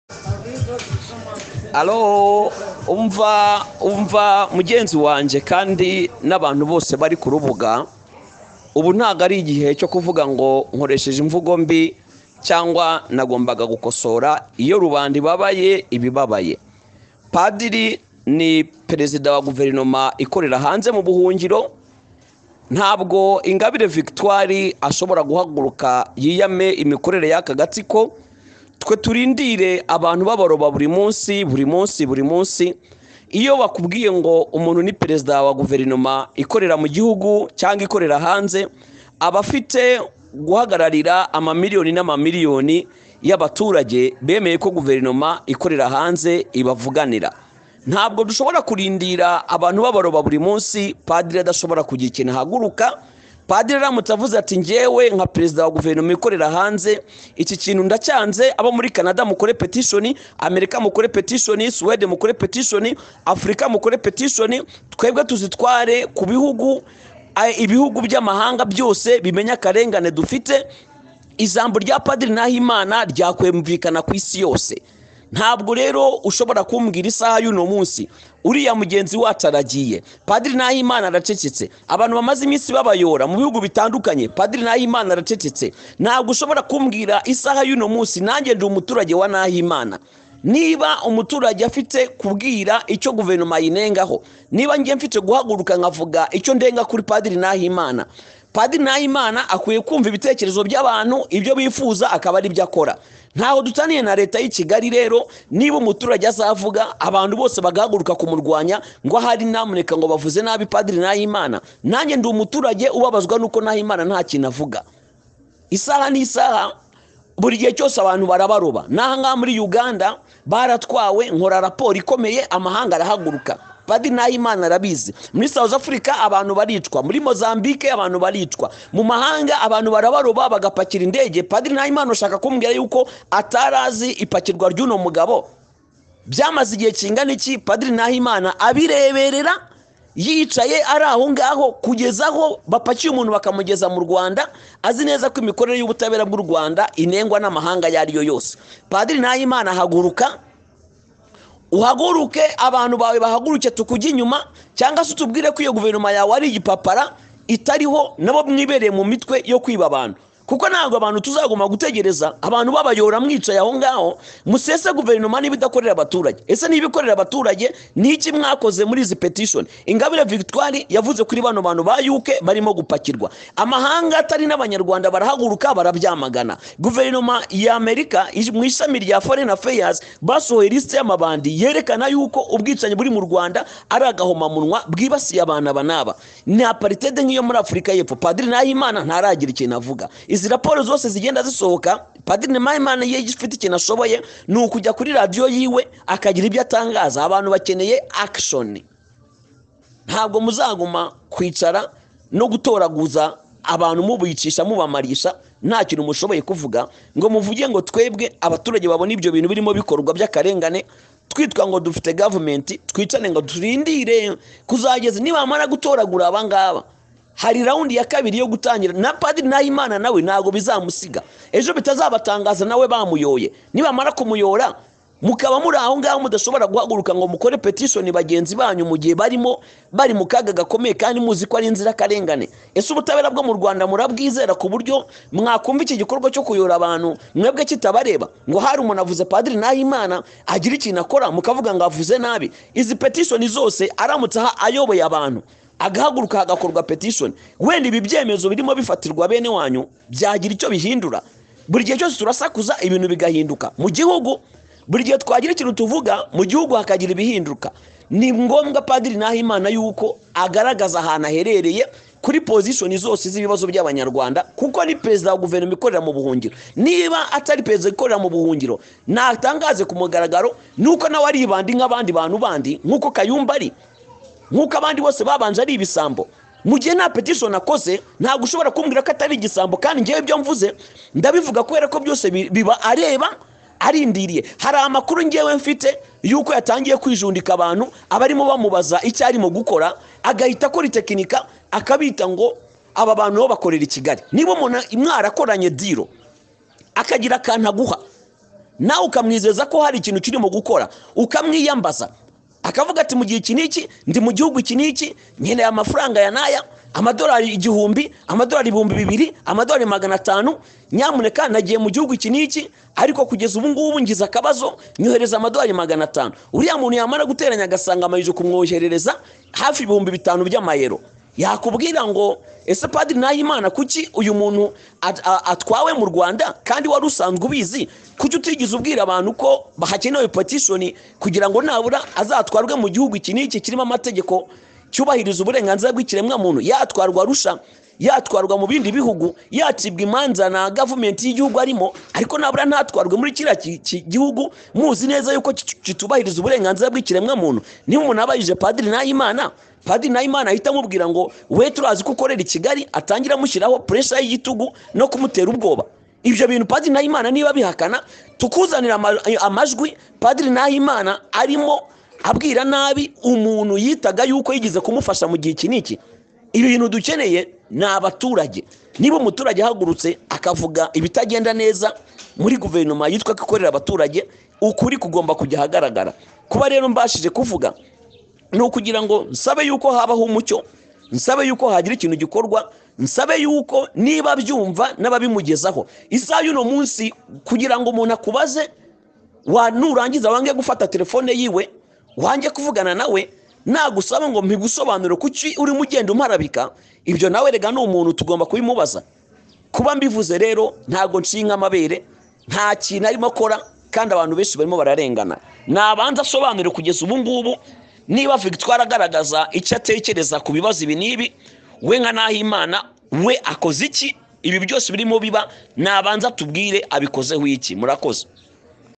NGO NAHIMANA NAFUNGE INGIRWA GVT NIBA ATAVUGIRA RUBANDA!. IYO UMUNENZE AGUSHINJA GUKORERA KIGALI🤷‍♂️
BIRAKAZE